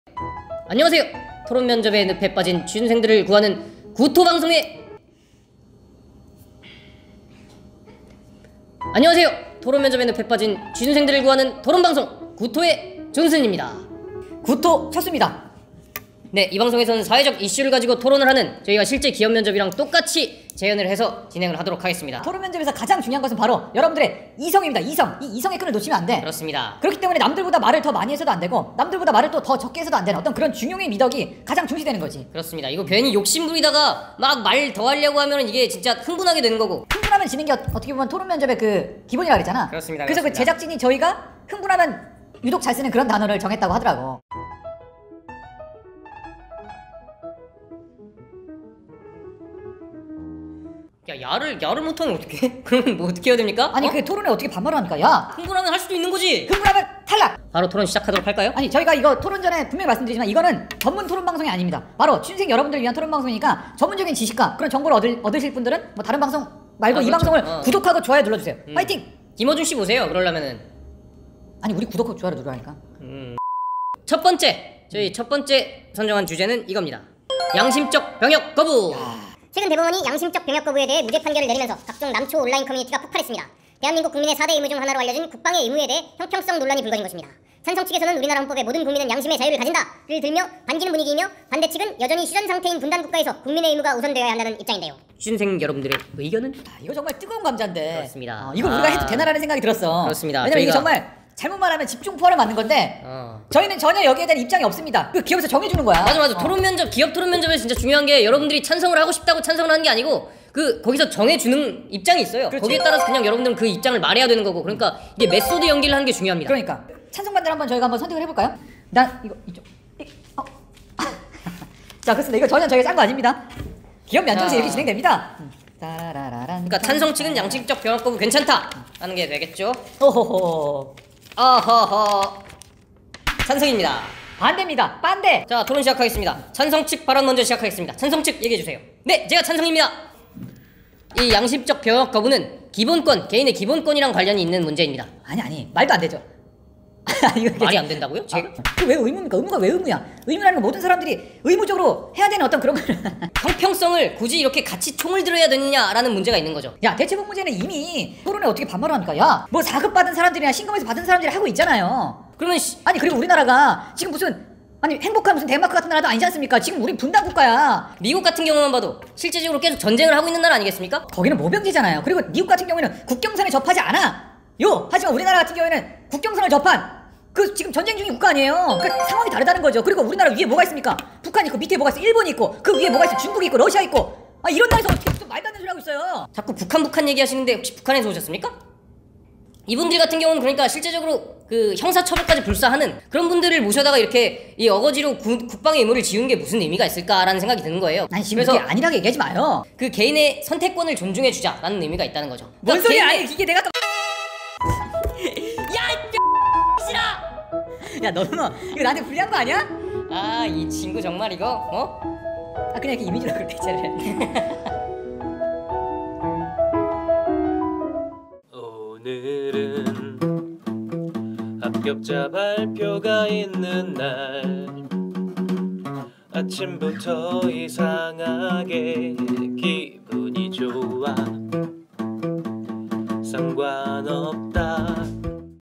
토론면접 기출 01ㅣ양심적 병역거부ㅣ대기업 공기업 다 나옴
토론면접 꿀팁 대방출(?)